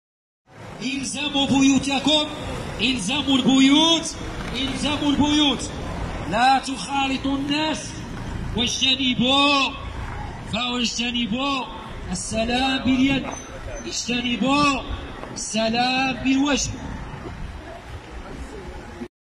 عاجل إلزموا بيوتكم...الشرطة بمدينة العروي تخاطب الناس عبر مكبر الصوت aroui nhara
الشرطة المغربية تتجول بالشارع لمخاطبة الناس بإلزامهم البيوت وعدم المصافحة وتجنب مخالطة الناس